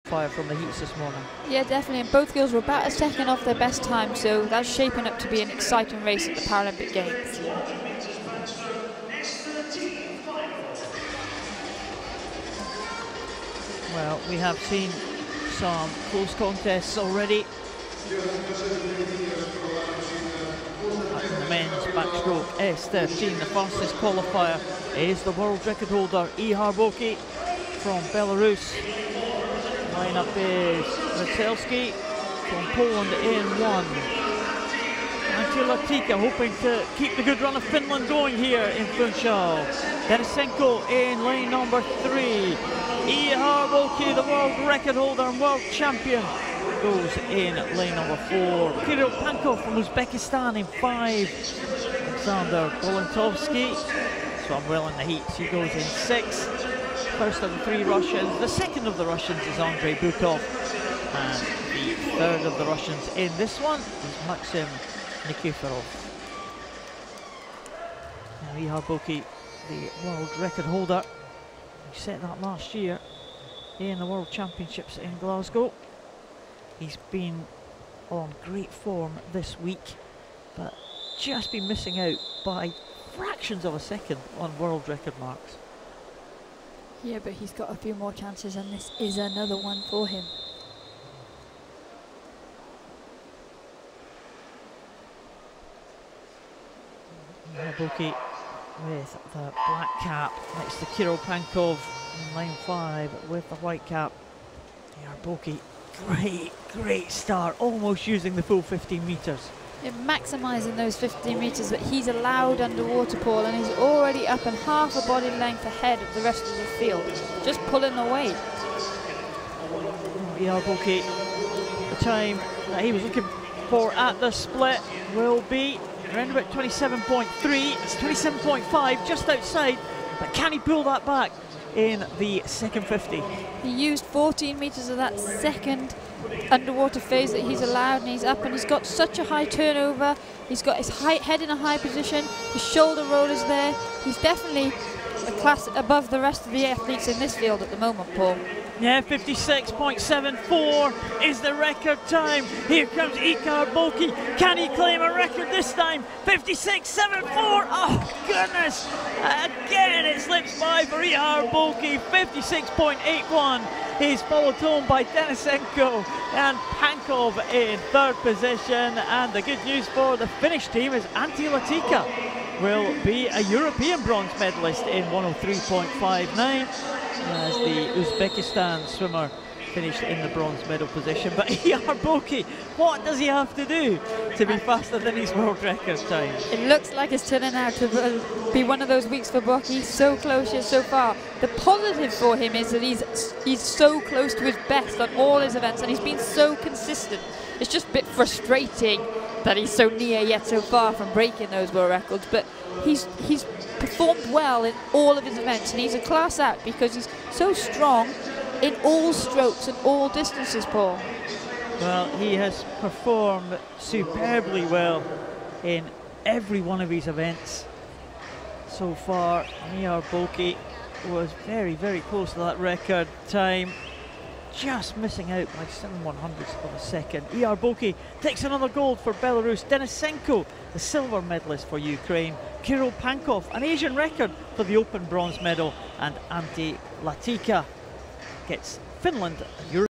Men's 100m Backstroke S13 | Final | 2016 IPC Swimming European Open Championships Funchal
Gold: BOKI Ihar Belarus
Silver: DENYSENKO Iaroslav Ukraine
Bronze: PANKOV Kirill Uzbekistan

Start list:
RZETELSKI Kamil Wiktor Poland
LATIKKA Antti Antero Finland
DENYSENKO Iaroslav Ukraine
BOKI Ihar Belarus
PANKOV Kirill Uzbekistan
GOLINTOVSKII Aleksandr Russian Federation
BUKOV Andrei Russian Federation
NIKIFOROV Maksim Russian Federation

453 athletes from 52 countries will compete in the 2016 IPC Swimming European Open Championships, the fourth edition of the competition.